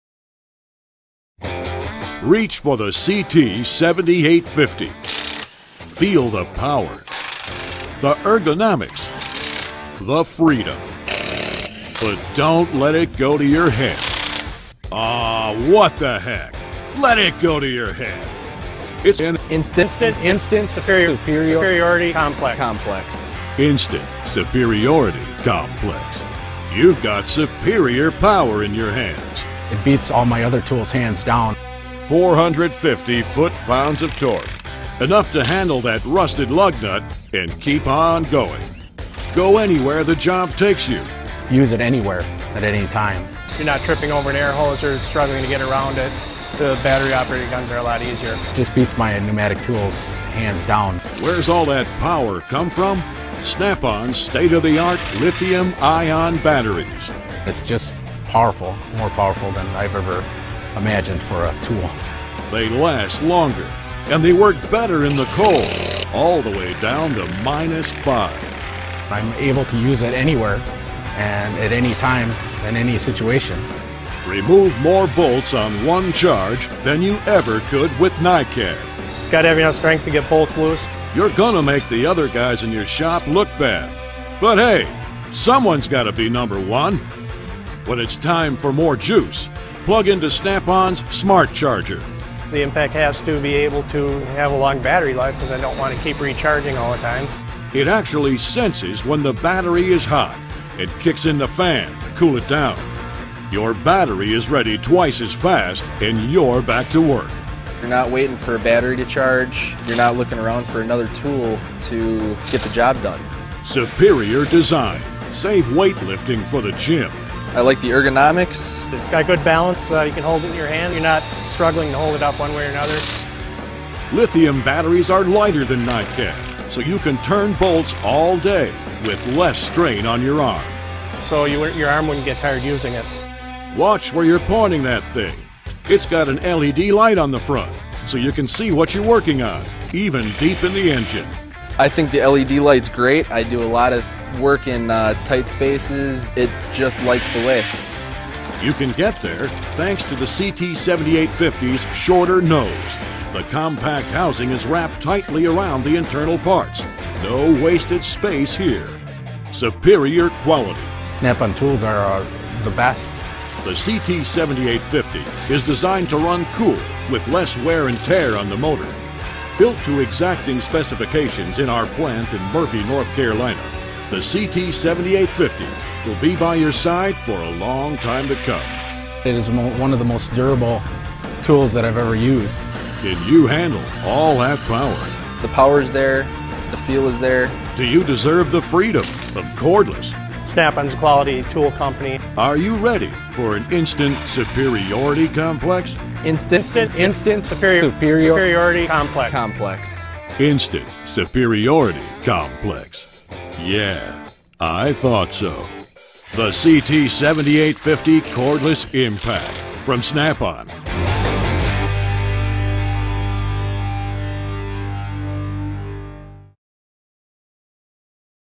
Masina de insurubat cu impact cu acumulator 18V CT 7850 - Snap-On
- Tip acumulator: Li-ion 3.0Ahr
- Capacitate acumulator: 18V
- Capacitate mandrina 13 mm
- Moment torsiunein. lbs. (Nm): 700 (949)
- Trepte: 22
- Nr. viteze: 2
- Turatie mers in gol: 1.800
- Timp incarcare (minute): 45
- Lungime, inch (mm): 9.2 (234)
- Greutate, lbs. (kg): 6.9 (3.1)
- Acumulator (buc.): CTB7185 (2)
- Cutie pentru transport: CTTOTEA
- Incarcator: CTC720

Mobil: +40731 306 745
Adresa noastra: Str. Stan Vidrighin nr.4, Timisoara, Timis